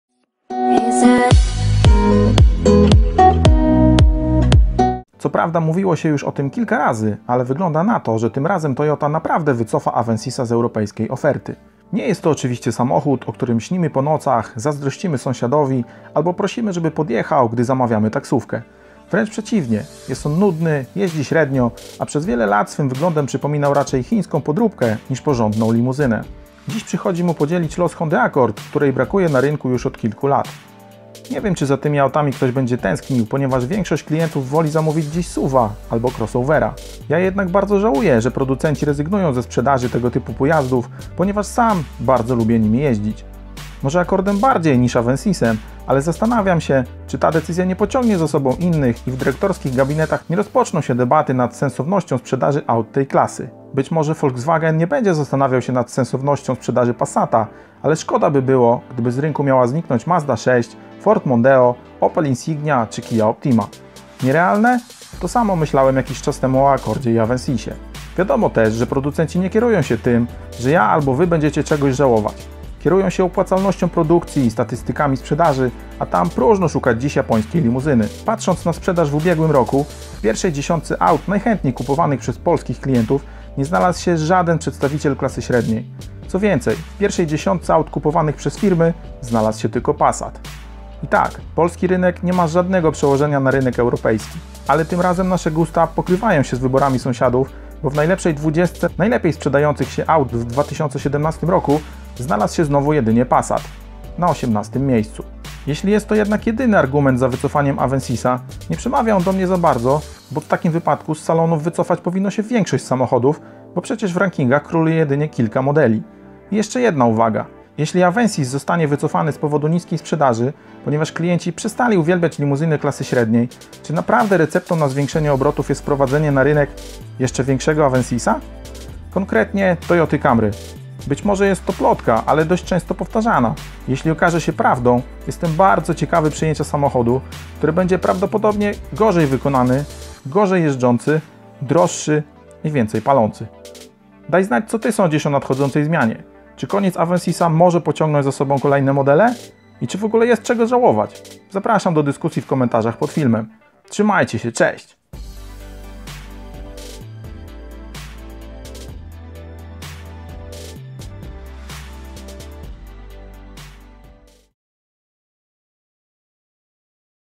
KONIEC TOYOTY AVENSIS !
Toyota Avensis przechodzi powoli do historii.
Jej następcą prawdopodobnie zostanie Toyota Camry.
Czy jest to dobry ruch japońskiego producenta? I co on oznacza dla klasy średniej?

Porozmawiajmy :)